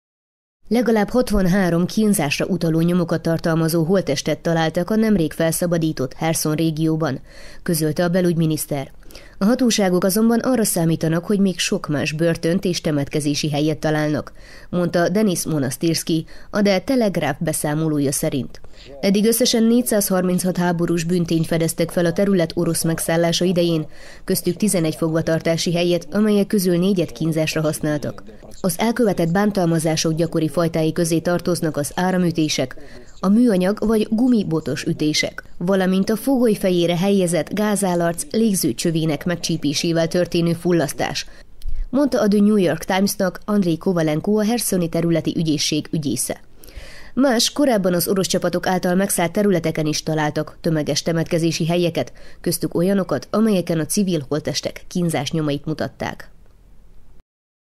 Tv21 Ungvár - Több tucat megkínzott áldozat holttestét találták meg Herszonban
Legalább 63, kínzásra utaló nyomokat tartalmazó holttestet találtak a nemrég felszabadított Herszon régióban – közölte a belügyminiszter. A hatóságok azonban arra számítanak, hogy még „sok más börtönt és temetkezési helyet” találnak – mondta Denisz Monasztirszkij a The Telegraph beszámolója szerint. Eddig összesen 436 háborús bűntényt fedeztek fel a terület orosz megszállása idején, köztük 11 fogva tartási helyet, amelyek közül négyet kínzásra használtak. Az elkövetett bántalmazások gyakori fajtái közé tartoznak az áramütések, a műanyag vagy gumibotos ütések, valamint a fogoly fejére helyezett gázálarc légzőcsövének megcsípésével történő fullasztás – mondta a The New York Timesnak Andrij Kovalenko, a herszoni területi ügyészség ügyésze. Más, korábban az orosz csapatok által megszállt területeken is találtak tömeges temetkezési helyeket, köztük olyanokat, amelyeken a civil holttestek kínzás nyomait mutatták.